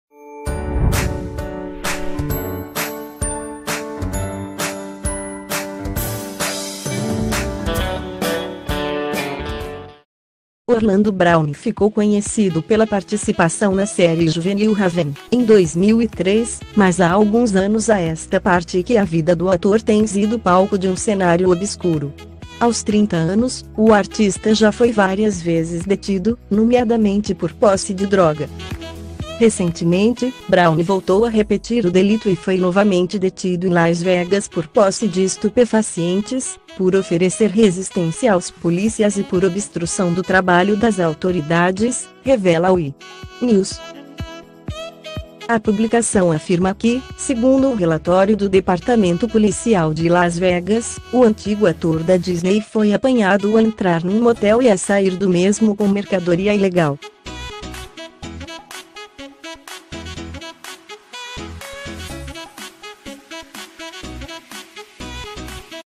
Ator da Disney, Orlando Brown, volta ser detido por posse de droga
Orlando Brown ficou conhecido pela participação na série juvenil 'Raven', em 2003, mas há alguns anos a esta parte que a vida do ator tem sido palco de um cenário obscuro. Aos 30 anos, o artista já foi várias vezes detido, nomeadamente por posse de droga.
[time]Recentemente, Brown voltou a repetir o delito e foi novamente detido em Las Vegas por posse de estupefacientes, por oferecer resistência aos polícias e por obstrução do trabalho das autoridades, revela o E! News.